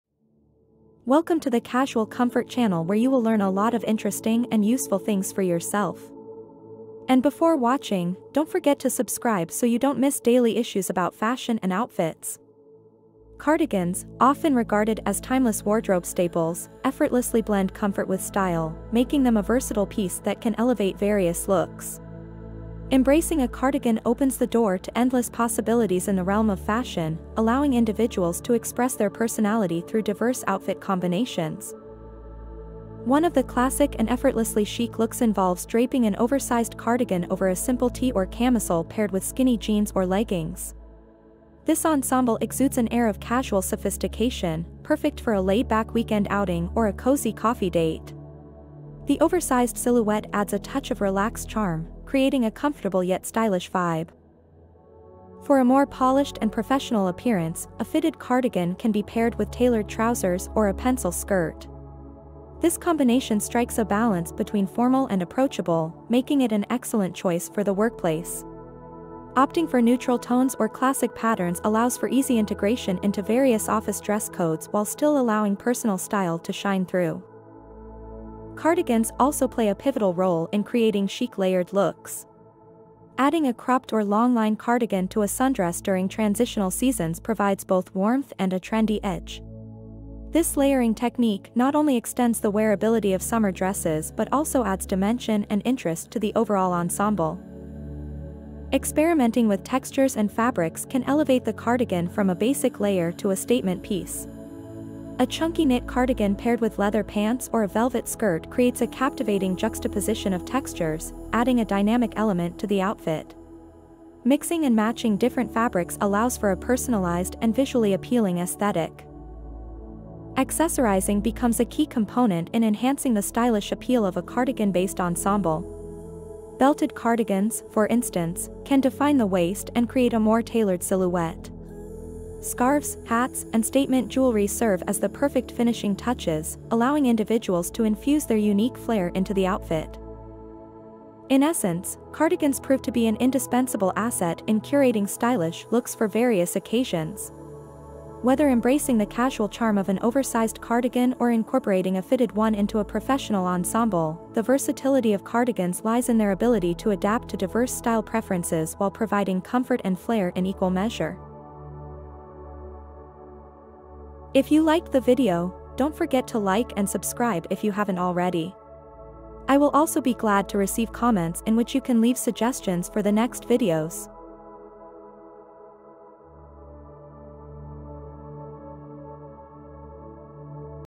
CARDIGAN | How to style cardigans
Unlock the magic of cardigans in this fashion journey! From casual elegance to office chic, we've curated a stunning collection of stylish looks that transform the humble cardigan into a versatile fashion statement. Join us as we explore outfit ideas, layering techniques, and smart accessorizing to turn your cardigan into the hero of your wardrobe. Elevate your style game effortlessly with these fabulous cardigan-inspired ensembles. Embrace the coziness without compromising on flair. It's time to redefine casual sophistication!